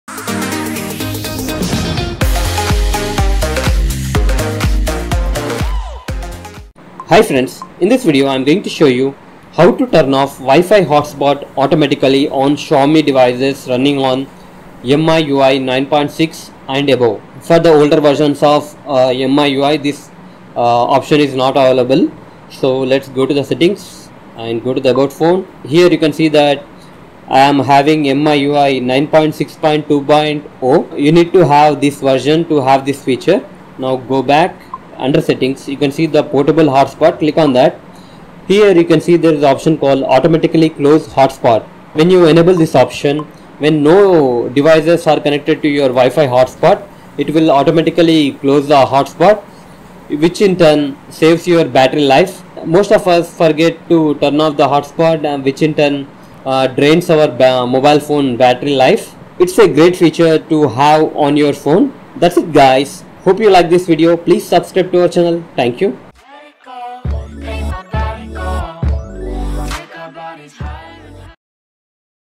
Automatically close Wifi Hotspot when no devices are connected in Xiaomi phones | Miui 9 / 10
Automatically close Wifi Hotspot when no devices are connected in Xiaomi phones running on  Miui 9 / 10 . This feature is available on devices running MIUI 9.6.2.0 and above.If your phone doesn't have this feature update your phone to MIUI 9.6.2.0 and above version.
 BY Automatically closing the Mobile hotspot we save the phone battery from discharging. This feature should be present on every other manufacture in the near future.
Steps to Turn-on " Automatically close Hotspot " feature in Xiaomi phones
1. Go to settings - Portable Hotspot
2. Enable " Automatically Close Hotspot "
That's it